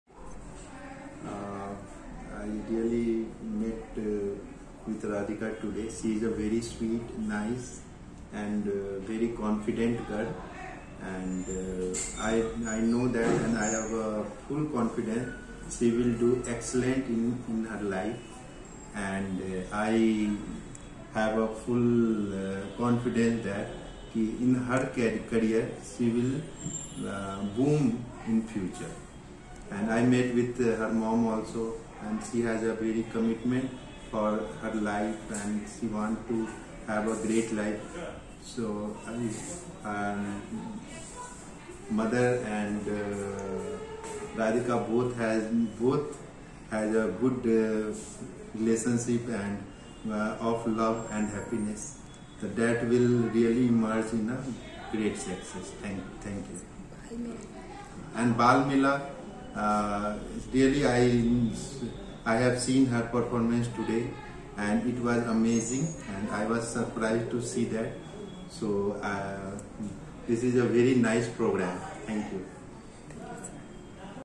भारतीय खाद्य निगम, क्षेत्रीय कार्यालय असम के उप महाप्रबंधक (लेखा) जय प्रकाश जी ने इस अवसर पर
भारतीय खाद्य निगम, क्षेत्रीय कार्यालय असम के उप महाप्रबंधक (लेखा) जय प्रकाश जी ने इस अवसर पर littlecoronawarriors को बधाई दी और बाल मेला टीम को 2021 के सफल आयोजन के लिये अग्रिम शुभकामना.